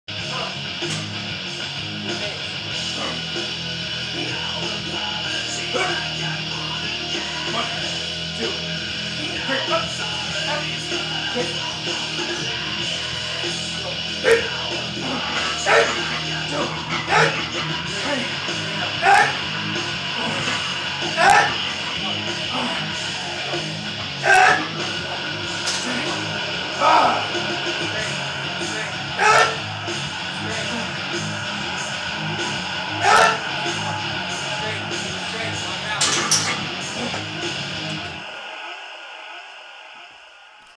Incline Bench Press 310x8 | Building Upper Chest & Pressing Strength | Strength Training
In this video, I hit 310x8 on the incline bench press, focusing on building upper chest strength and improving overall pressing power. The incline press targets the upper portion of the chest, shoulders, and triceps, helping to create a well-rounded, strong upper body. Watch as I break down proper form, key tips for maximizing muscle activation, and how to integrate incline bench presses into your routine to improve your pressing strength and upper chest development.